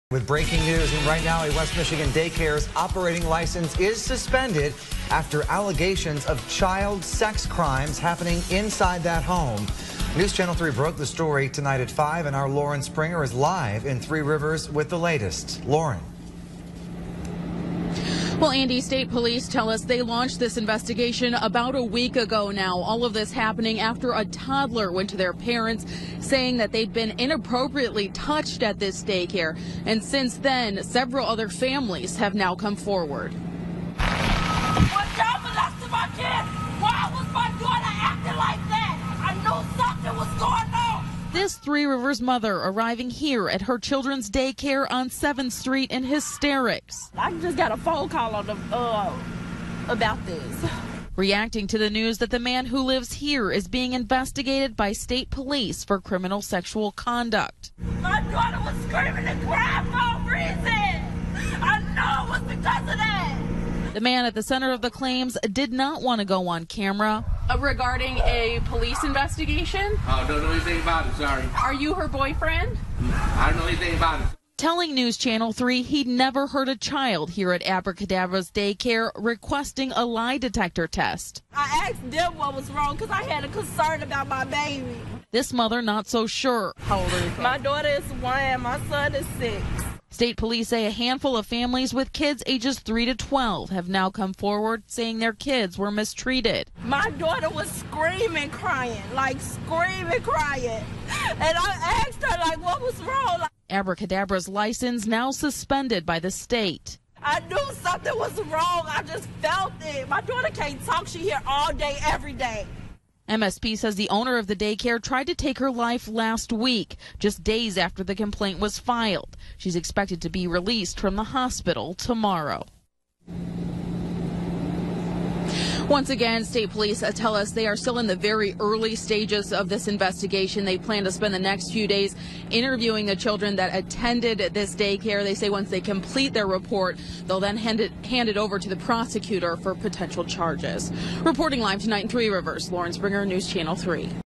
Daycare license suspended following accusation of child sex crimes
THREE RIVERS, Mich. (NEWSCHANNEL 3) - A West Michigan daycare's license has been suspended after allegations of child sex crimes inside the home. State Police tell us they launched this investigation last week after a toddler went to their parents, saying they were touched at the daycare.Now other families are coming forward as well.While Newschannel 3 was on the scene Tuesday, a Three Rivers mother arrived here at her children's daycare on 7th Street in hysterics."I just got a phone call about this," she said.She was reacting to the news that the man who lives there is being investigated by State Police for criminal sexual conduct."My daughter was screaming and crying for a reason, I know it was because of that," the mother said.The man at the center of the claims did not want to go on camera. But he told Newschannel 3 that he'd never hurt any child at Abracadabra's Day Care in Three Rivers, and requested a lie detector test.At least this mother, however, was not so sure."My daughter is 1, my son is 6," she said.State Police say a handful of families with kids ages 3 to 12 have now come forward saying their children were mistreated."My daughter was screaming crying, like screaming crying and I asked her what was wrong," a mother said.Abracadabra's license has now been suspended by the state while the investigation continues.The owner of the daycare tried to take her life last week, just days after the complaint was filed. She's expected to be released from the hospital Wednesday.To reiterate, the man accused has not been arrested. State Police tell us they'll spend the next several day interviewing kids. They then submit their report to the prosecutors office who will determine if there will be charges.